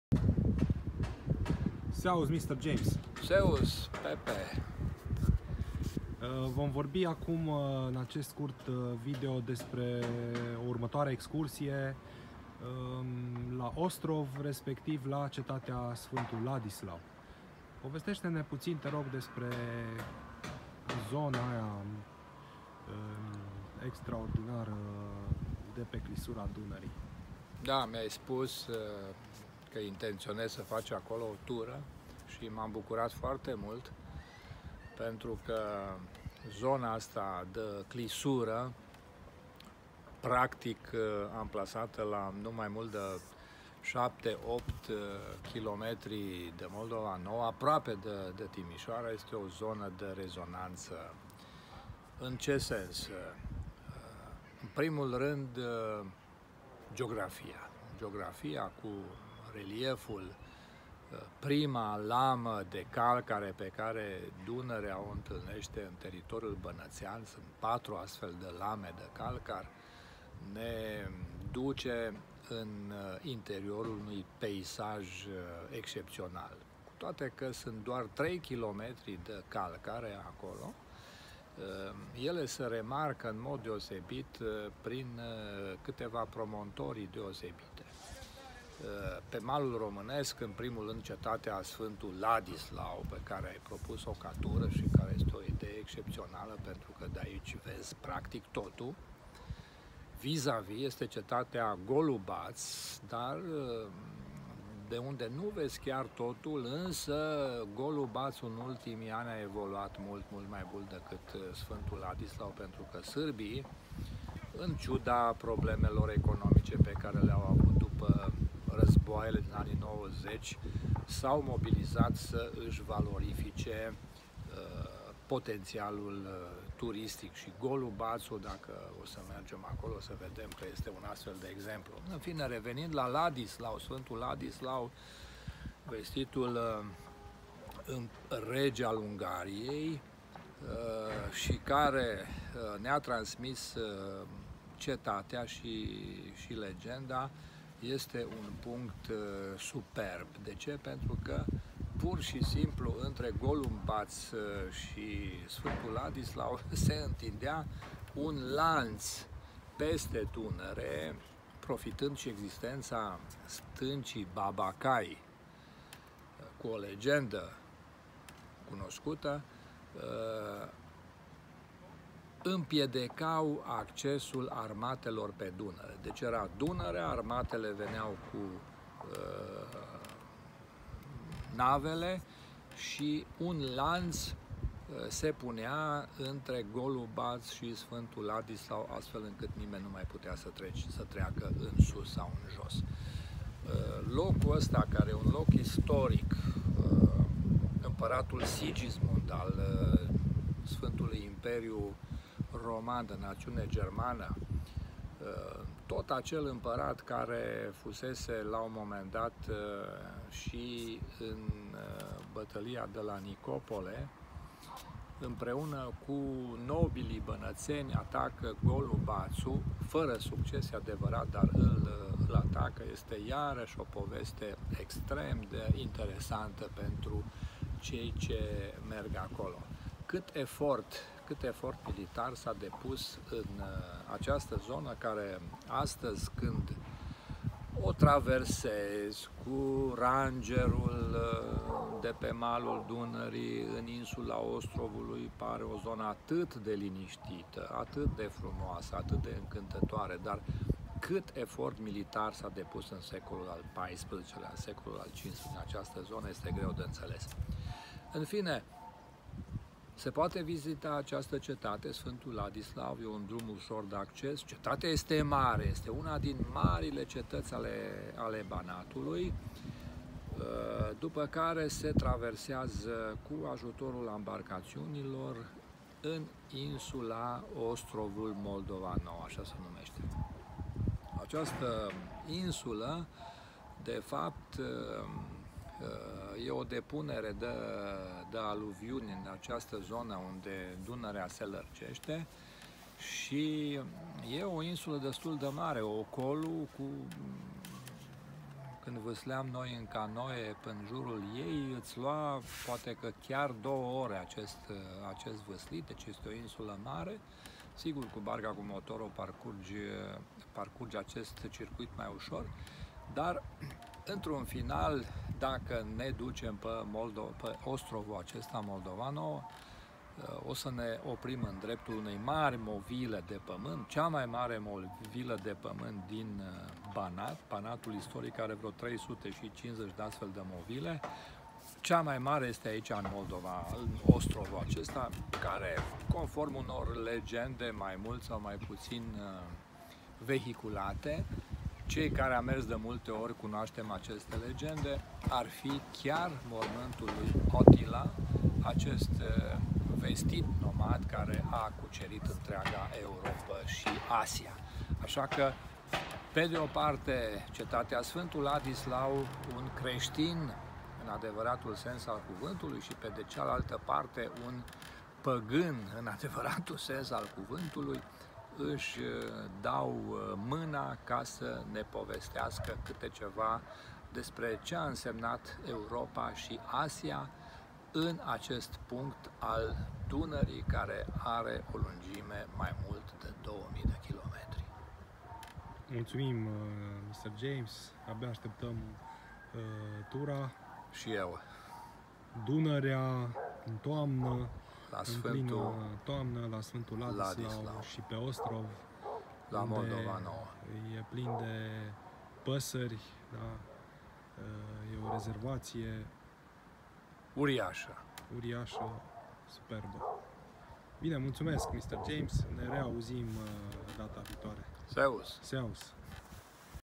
Equilibrium | Mister James | Cetatea Ladislav si Ostrov
Awesome Weekend cu Mister James in Clisura Dunarii. O cetate si o insula, poftiti si veniti cu noi!